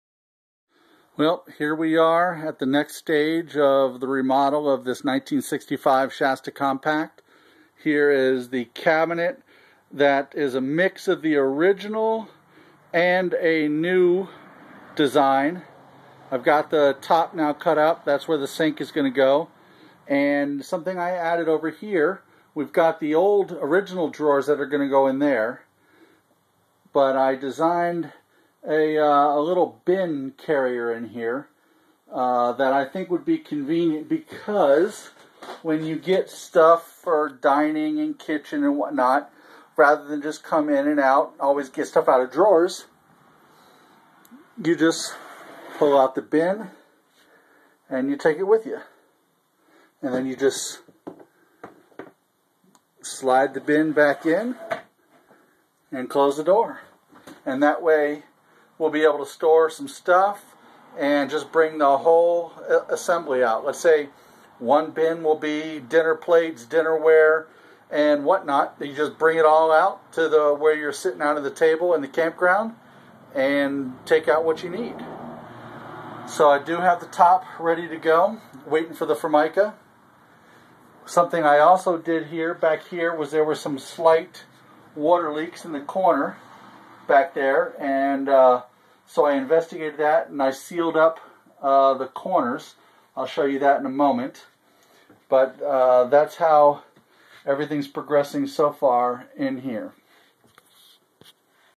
1965 Shasta Compact Part 3 ~ Cabinets ~ Vintage Trailer Restoration
Part 3 of the 1965 Shasta Compact Travel Trailer Repair & Renovation. Some custom cabinet mods...